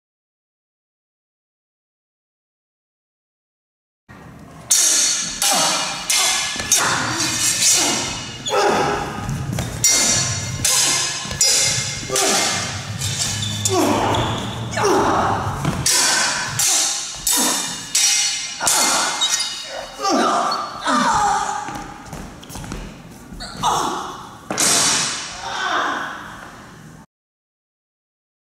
Jayme and Charlotte - Basic Broadsword 2
Jayme Wojciechowski and Charlotte Bjornbak perform their second basic broadsword fight at the Academy of Theatrical Combat.